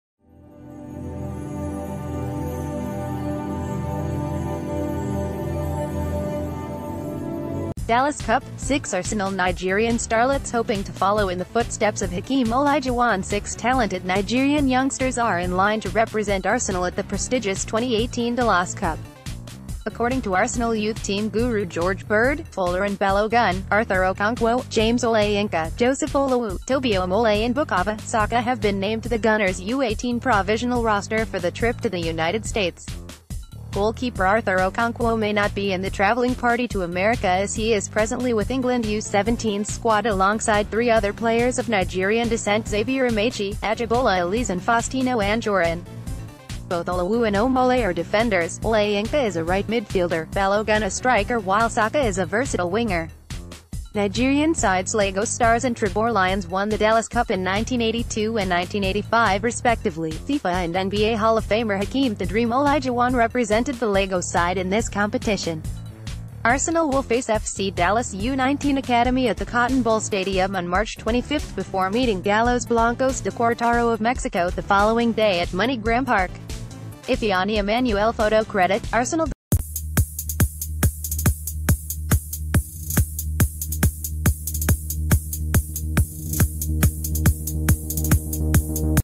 Breaking News | Dallas Cup
Dallas Cup : Six Arsenal Nigerian Starlets Hoping To Follow In The Footsteps Of Hakeem Olajuwon Six talented Nigerian youngsters are in line to represent Arsenal at the prestigious 2018 Dallas Cup. According to Arsenal youth-team guru Jeorge Bird, Folarin Balogun, Arthur Okonkwo, James Olayinka, Joseph Olowu, Tobi Omole and Bukayo Saka have been named to the Gunners U18 provisional roster for the trip to the United States. Goalkeeper Arthur Okonkwo may not be in the traveling party to America as...